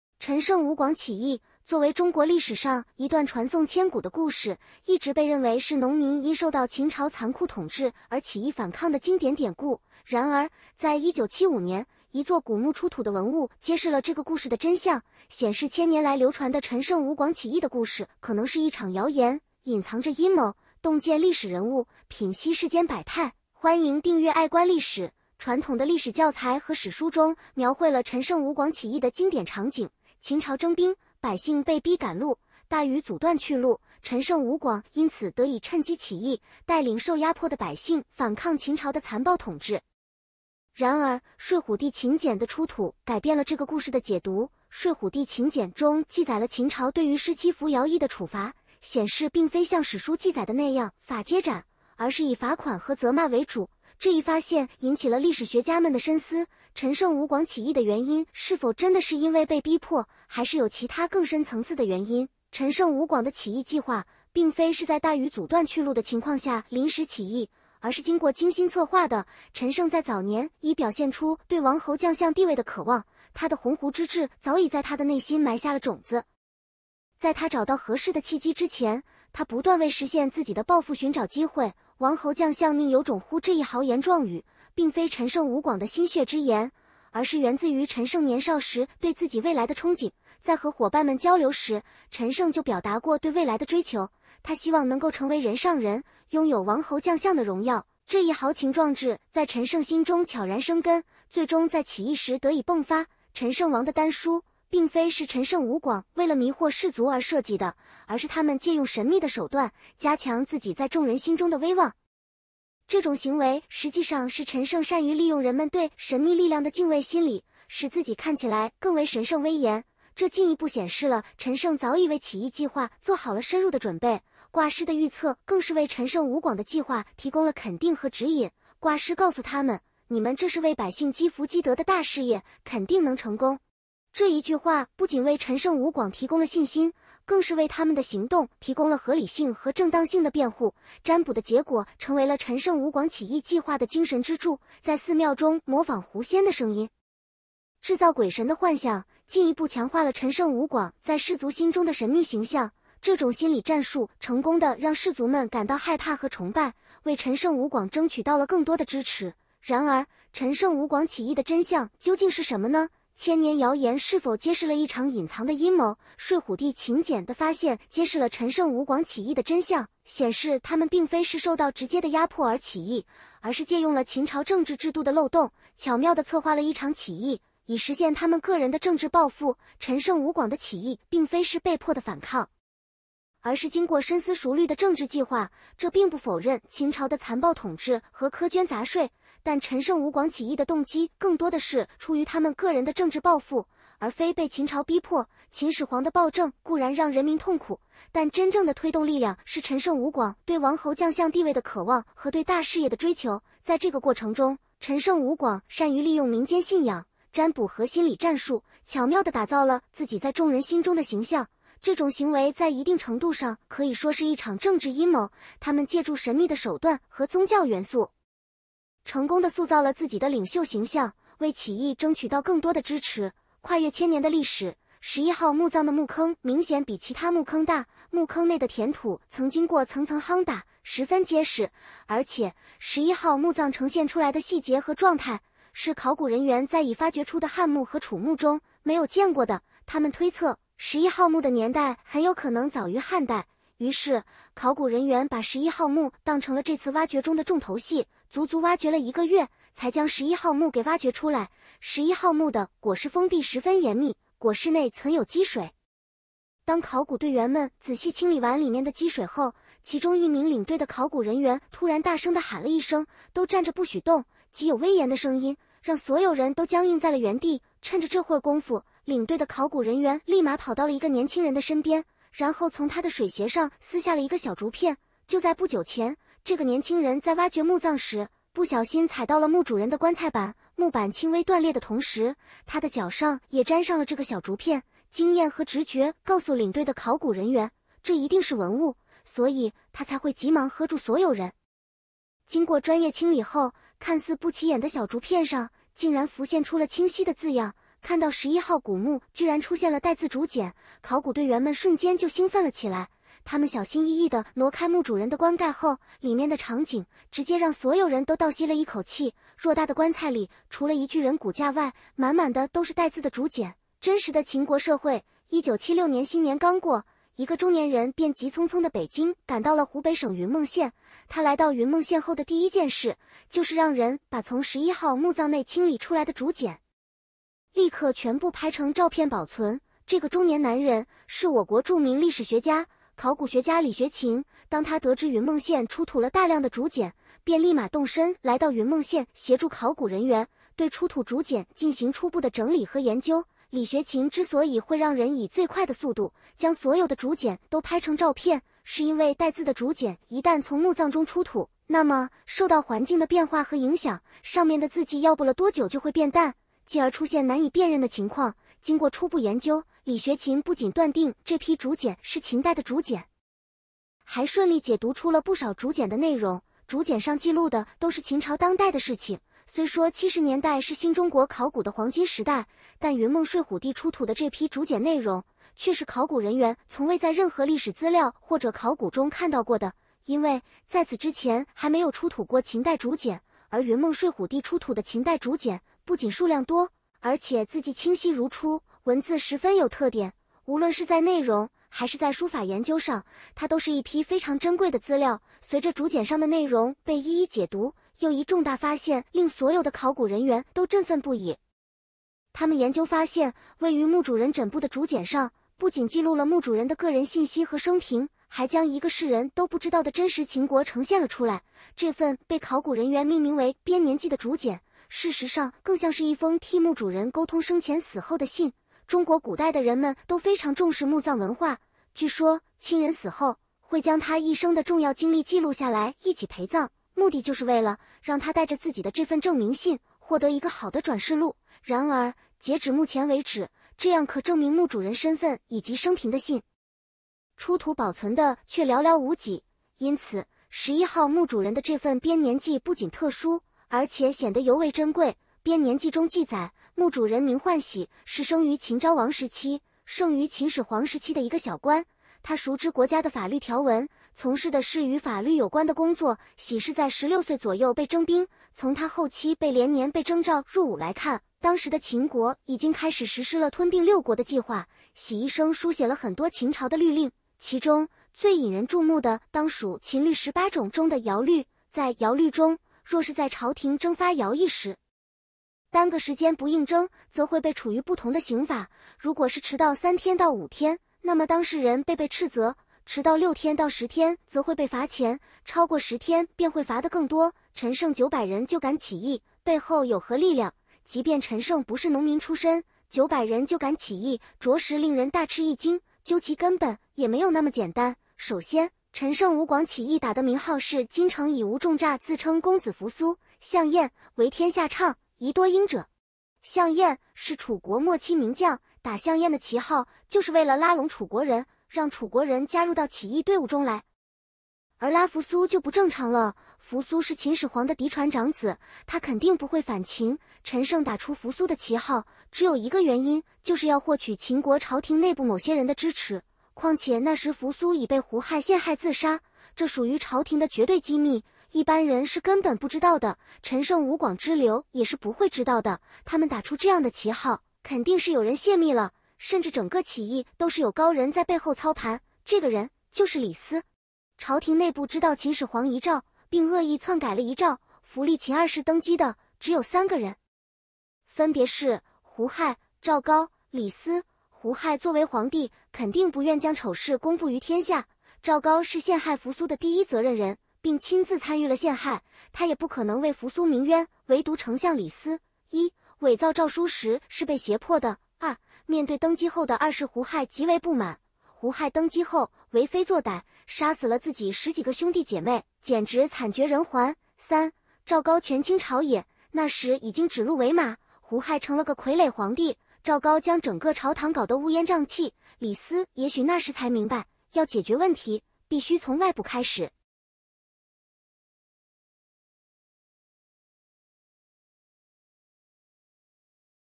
一件秦墓出土的文物，專家看完后直接呆住，陳勝吳廣起義真相終於揭開【愛觀歷史】古代
陳勝吳廣起義，作為中國歷史上一段傳頌千古的故事，一直被認為是農民因受到秦朝殘酷統治而起義反抗的經典典故。然而，在1975年，一座古墓出土的文物揭示了這個故事的真相，顯示千年來流傳的陳勝吳廣起義的故事可能是一場謠言，隱藏著陰謀。